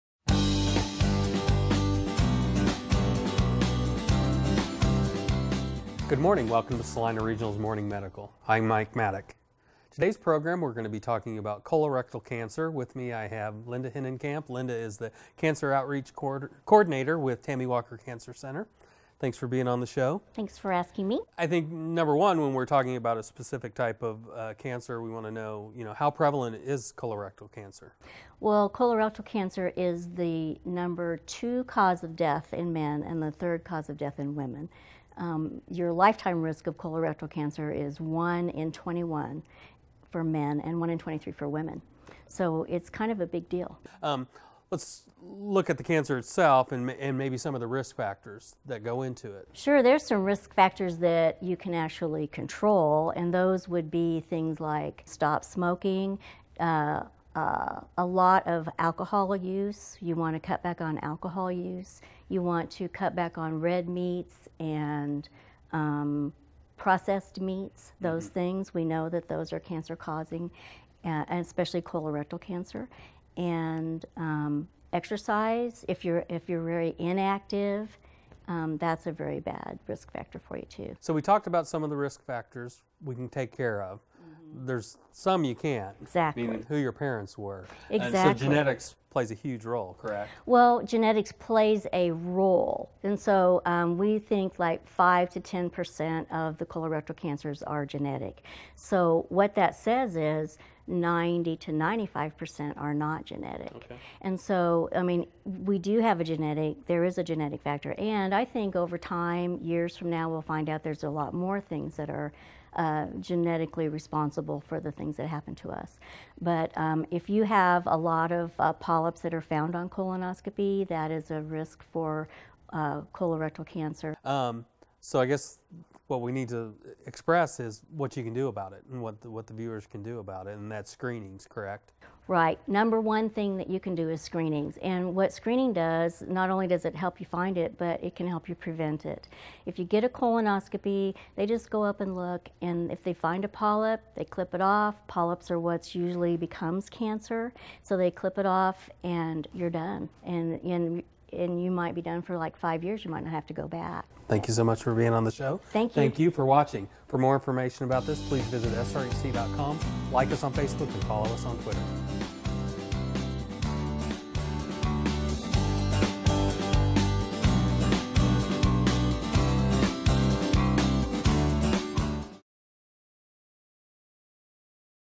Morning Medical | Colorectal Cancer – Linda Hinnenkamp, RN
90%-95% of colorectal cancers are due to controllable variables like smoking, diet and exercise. Linda Hinnenkamp, RN, Cancer Outreach Coordinator at Tammy Walker Cancer Center, shares some of those variables and what you can do to reduce your risk as well as monitor your colon health.